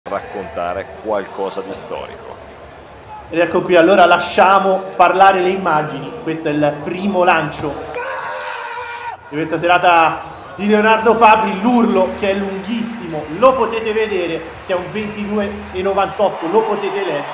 Leonardi Fabbri realizza il record italiano (22.98) a Bruxelles
Leonardi Fabbri si esalta al 'Memorial Van Damme' e timbra il record italiano del peso con 22,98 al primo lancio (+3 centimetri rispetto al primato realizzato in maggio a Savona), a un soffio dai 23 metri e a soli otto centimetri dal record europeo di Ulf Timmermann, sempre più vicino: battuto il tre volte campione olimpico Ryan Crouser (22,79).

Segui Atletica Italiana TV su Sportface: tv.sportface.it